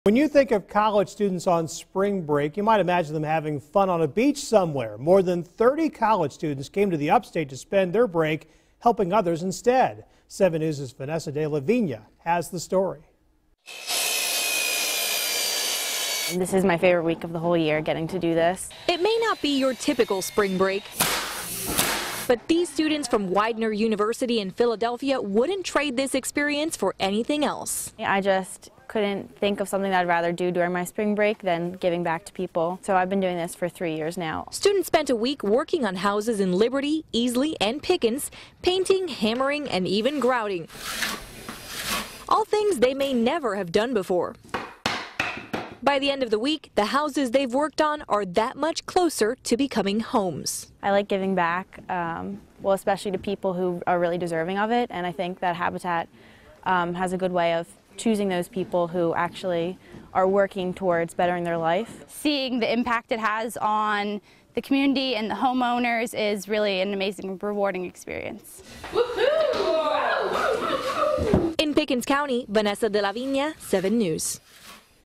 Students spend Spring Break helping others
College students spend Spring Break building houses in Pickens Co.